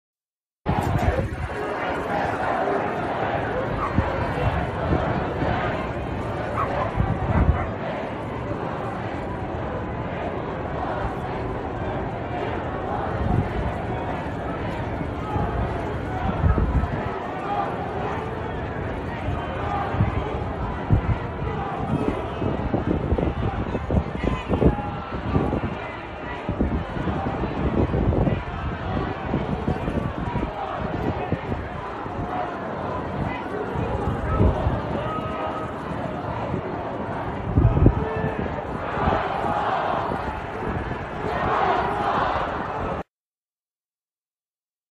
Chicagoan's come out in massive numbers in a show of solidarity for Ukraine
*peaceforUkraine